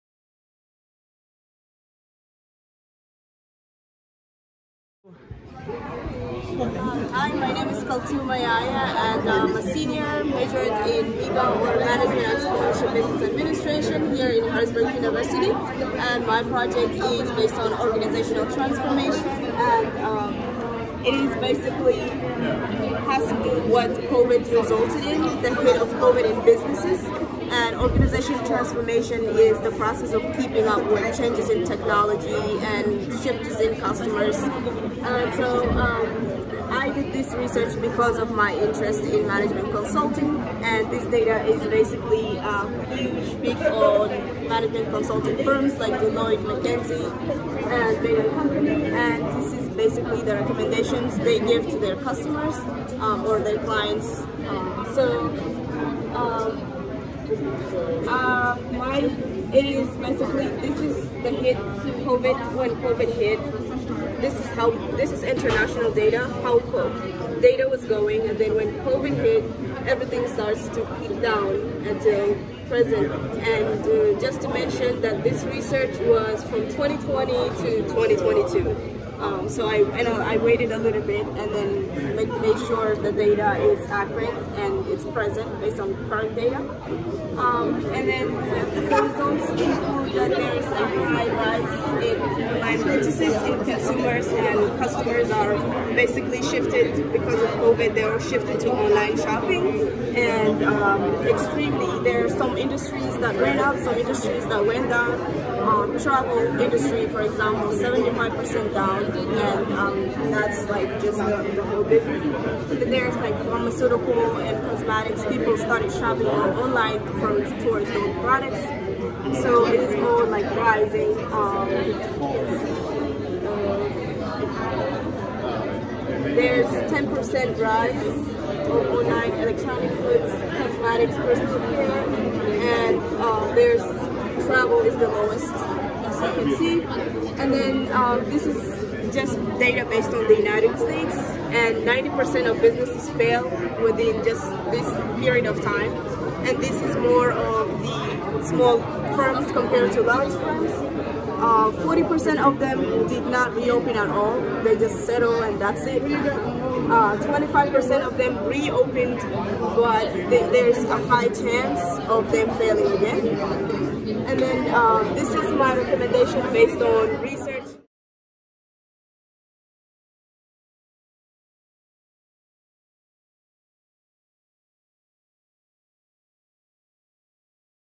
Management, Entrepreneurship, and Business Administration (MEBA) at HU
Check out this inspiring post about Harrisburg University's Management, Entrepreneurship, and Business Administration (MEBA) Bachelor of Science program! MEBA students like Senior Katouma Yaya are taught valuable skills such as strategic thinking, managing diverse workforces, leading team efforts, and negotiating successfully.

In the following video, Yaya shares her research on the impact of COVID-19 on businesses and offers recommendations for those who survived the pandemic.

At Harrisburg University, we are proud to offer an innovative and research-focused MEBA program that prepares students for successful careers in the modern job market.